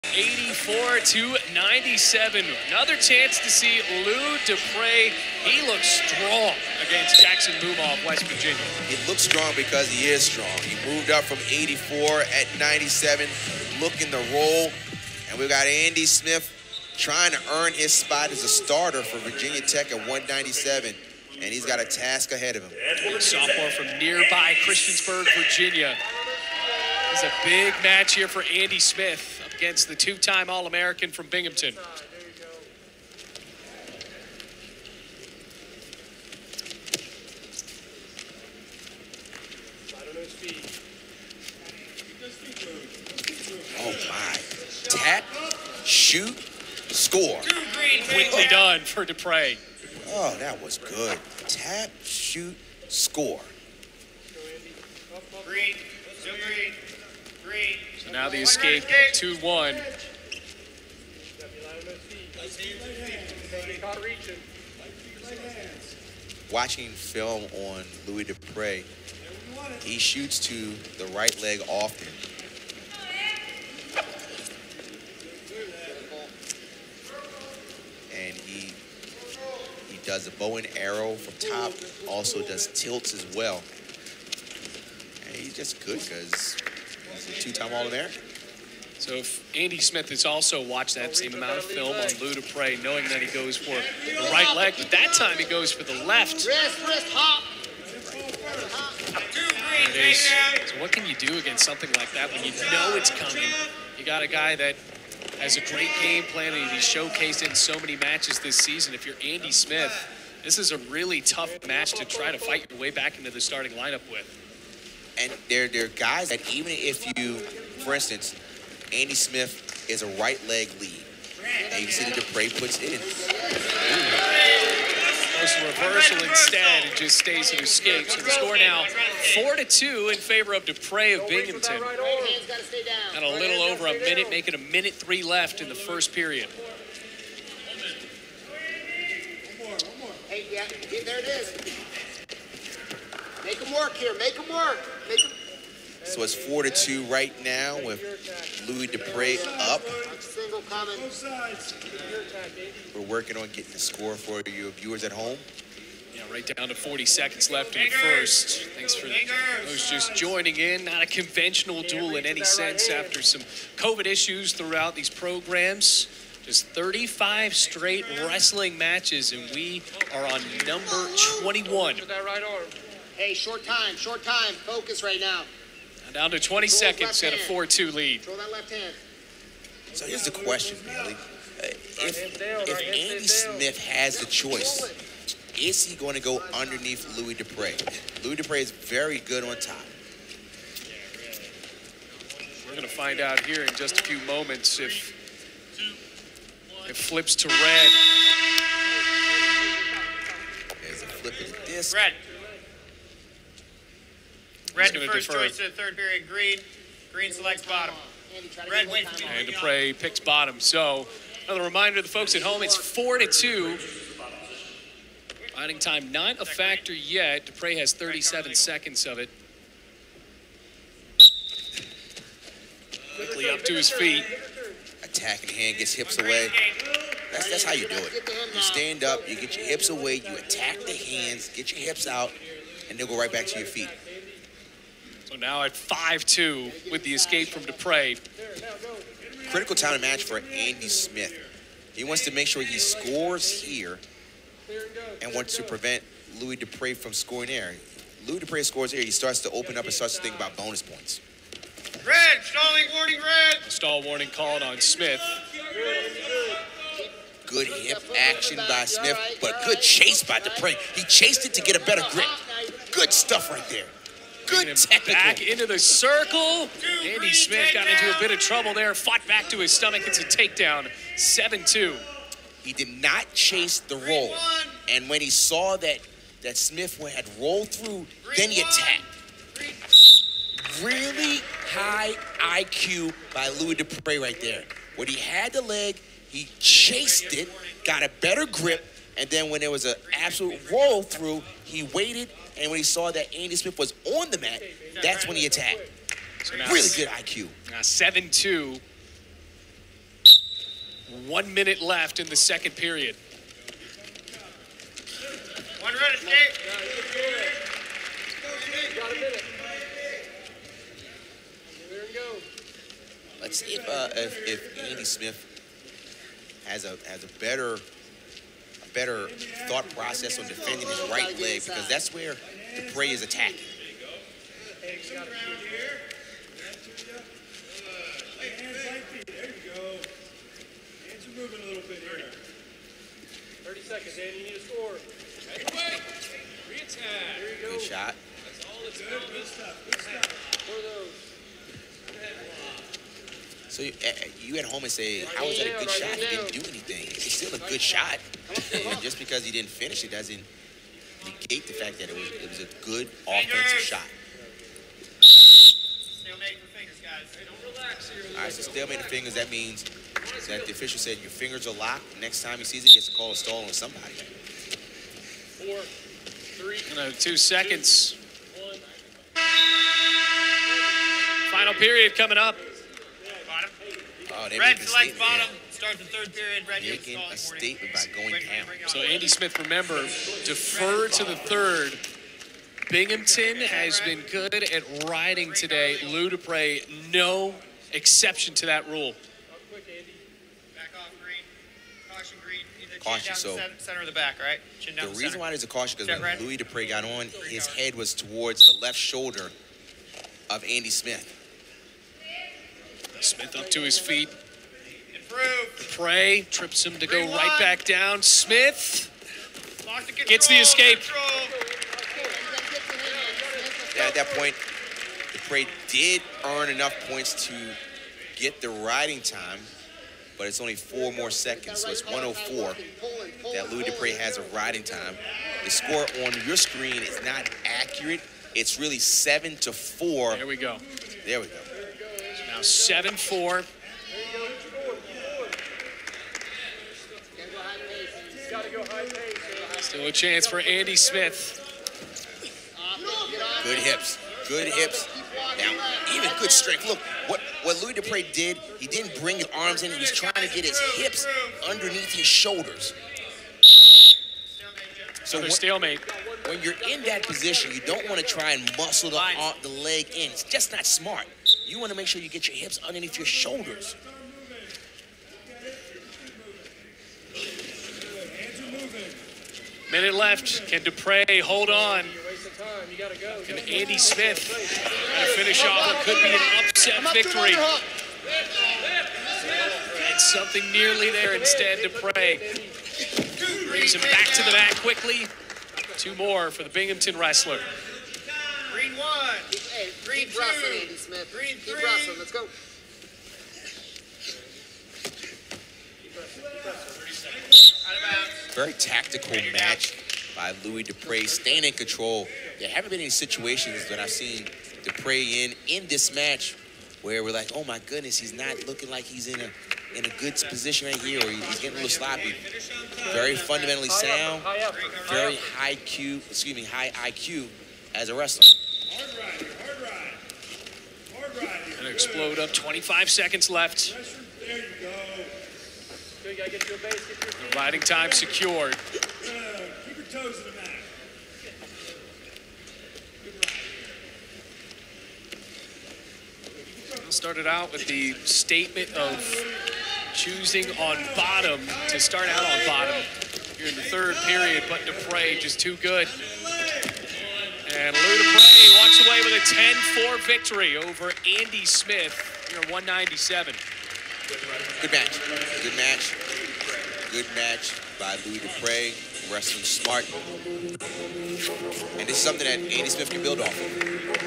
184lbs Lou DePrez (Binghamton) dec Andy Smith (Virginia Tech)
Virginia Tech vs Binghamton wrestling dual 2021/22
184lbs #12 Lou DePrez (Binghamton) vs Andy Smith (Virginia Tech)